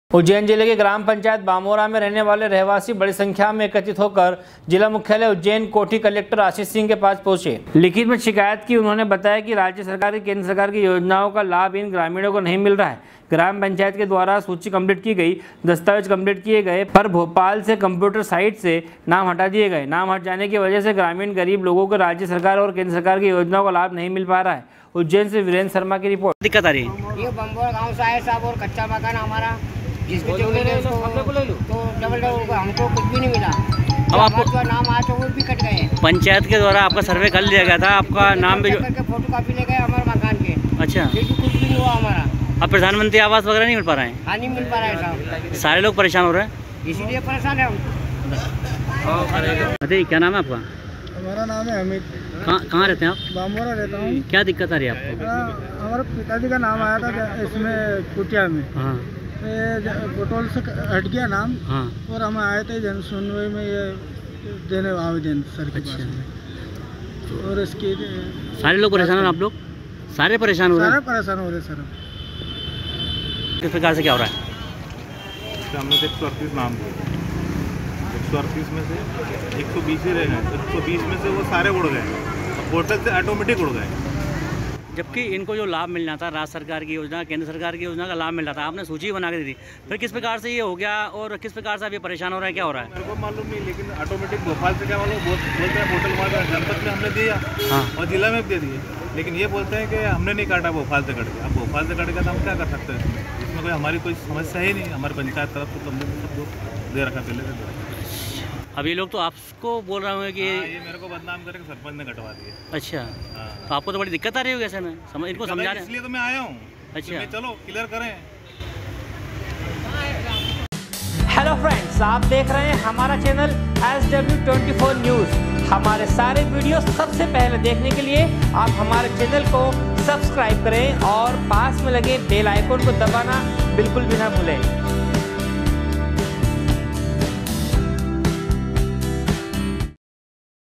SW 24 News Madhyapradesh
उज्जैन जिले के ग्राम पंचायत बामोरा में रहने वाले रहवासी बड़ी संख्या में एकत्रित हो कर जिला मुख्यालय उज्जैन कोठी कलेक्टर आशीष सिंह के पास पहुंच उज्जैन से विरेन्द्र शर्मा की रिपोर्ट